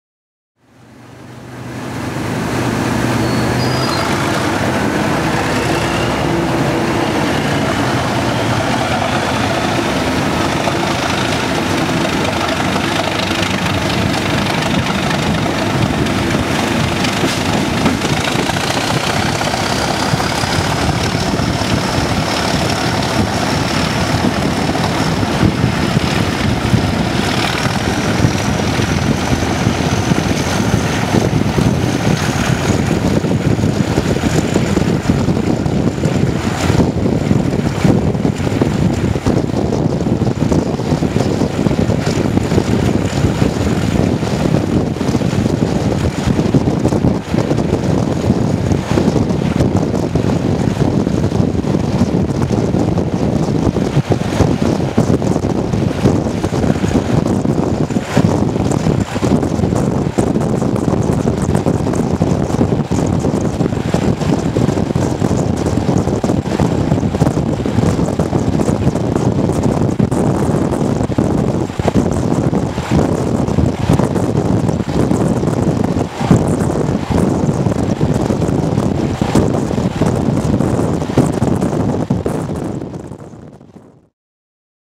Thrashing away from Cantley with DRS 37 405
Travelling on 2J80 14:55 Norwich to Lowestoft, we thrash away from Cantley at 15:08. On the rear is 37 425. Note the old semaphore signals, much of the Norwich to Lowestoft and Great Yarmouth lines still have semaphore signals, proving the old saying, "If it ain't broke, don't fix it".